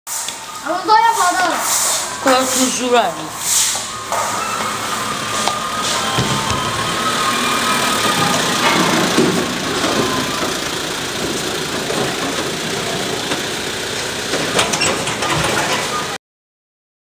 Sony【少年ㄟ環保心聲】-校園廢棄物=分類回收6
作者:屏東縣泰武國小 四年級 徐念宗

指導老師:林道君

作品說明:
舉手之勞做環保!校園乾淨又美麗!
垃圾分類好ok!資源回收不浪費!